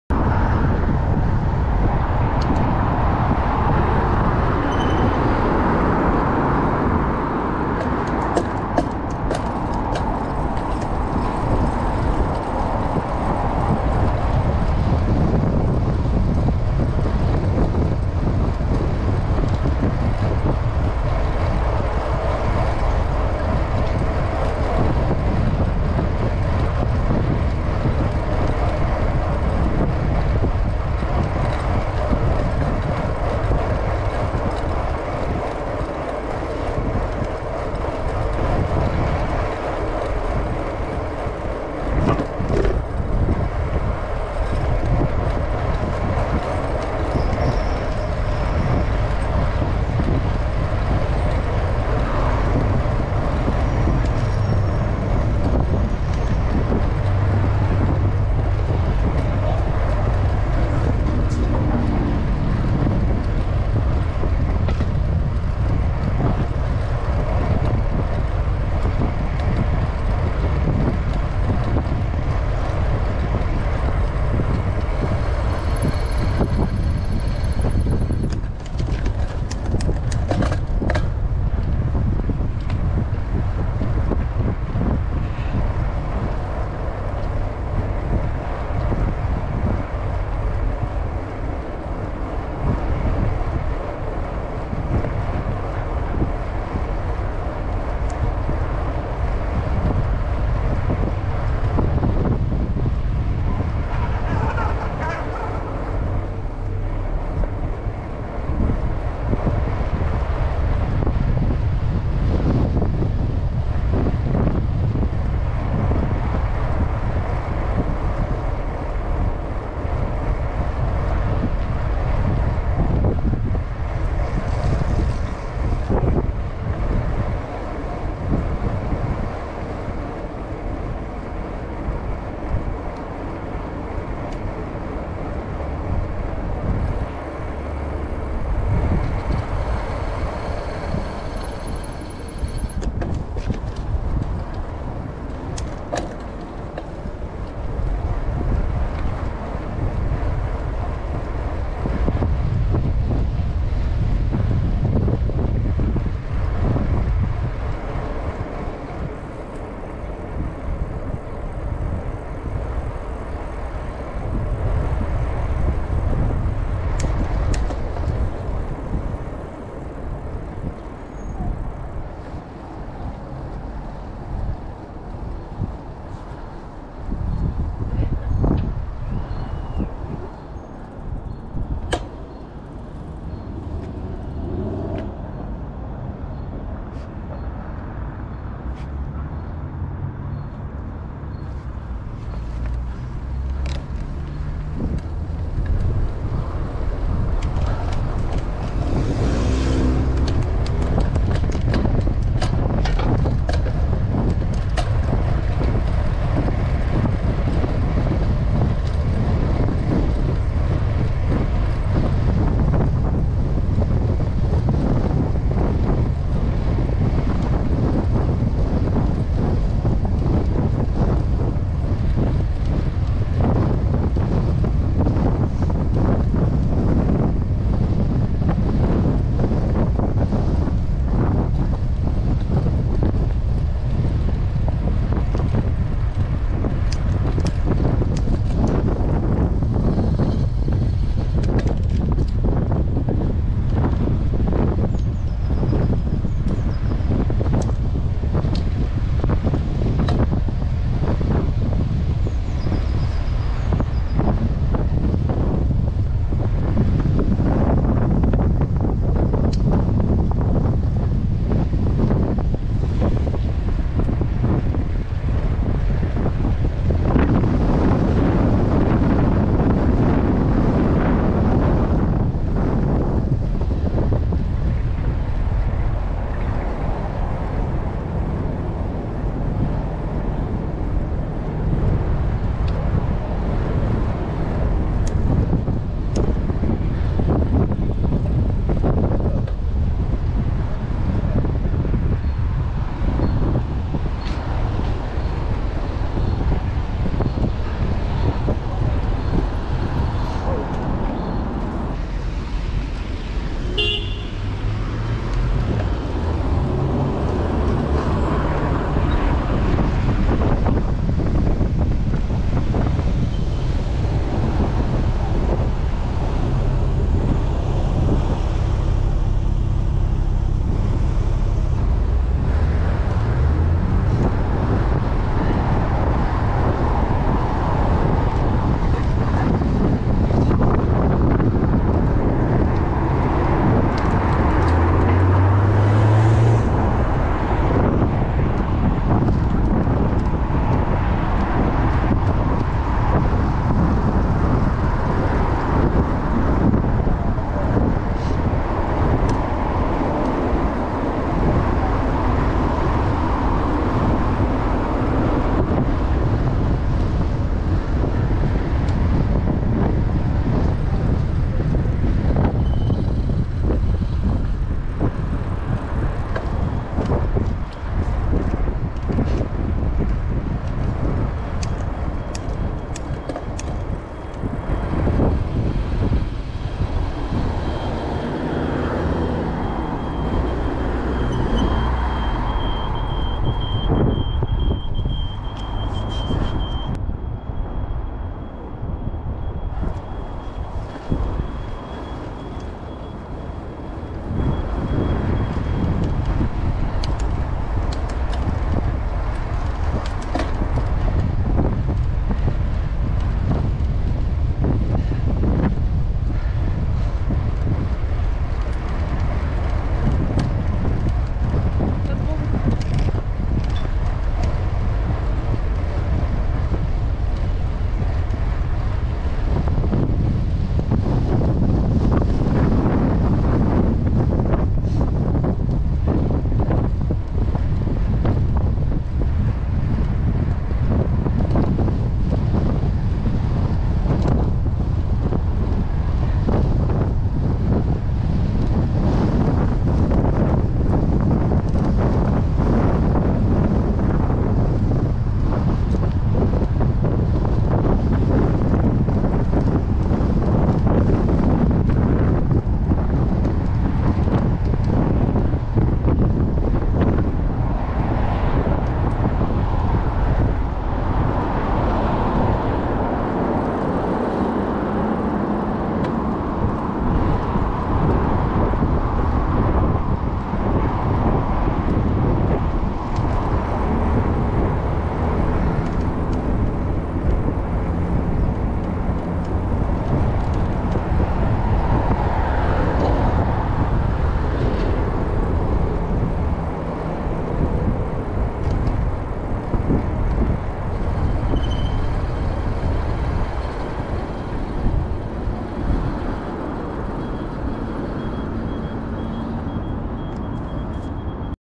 Садовническая и Болотная набережные после реконструкции
Поездка на велосипеде по Садовнической набережной, а также по участку Болотной набережной, которые были комплексно благоустроены.